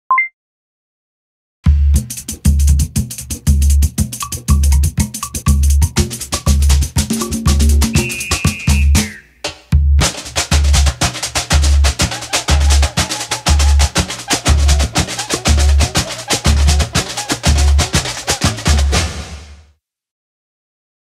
SONY - BLUETOOTH (Samba)
We use the Bluetooth symbol to be the starting point of the word Samba. The musical rhythms are formed on the screen in the same cadence of the music.